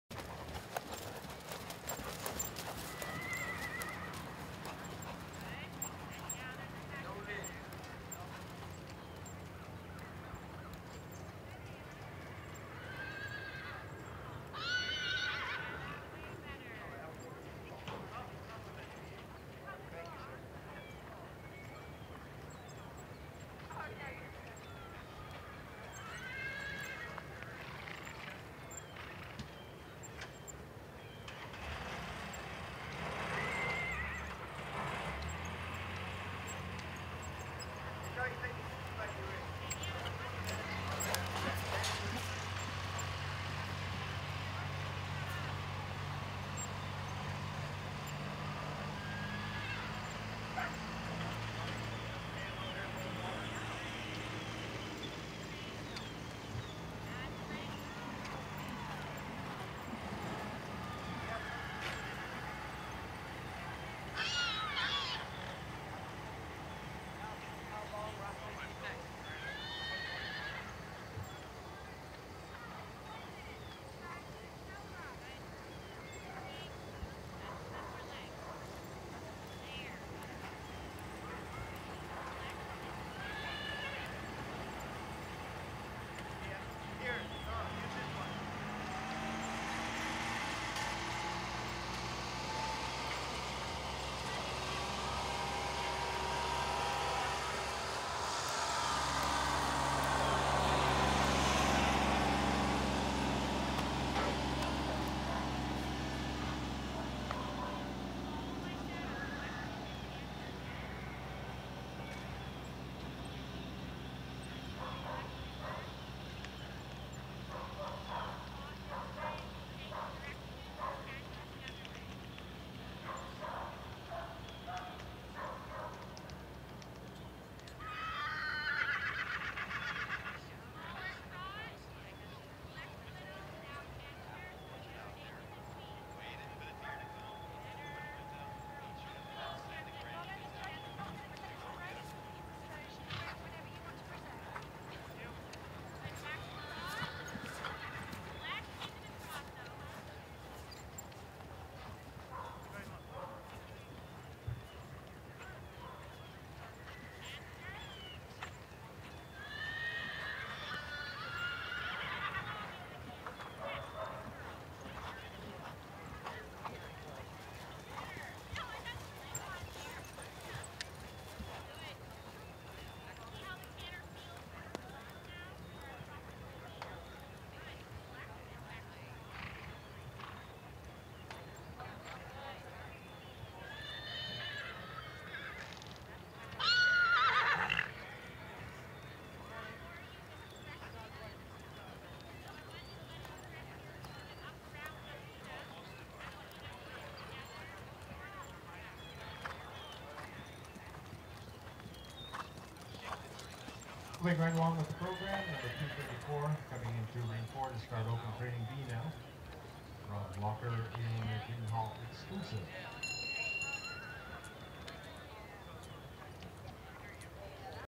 Samantha McCleerey Dressage - Fernhill First Choice
Dressage at The Grand Oaks Horse Trials with Fernhill First Choice and Samantha McCleerey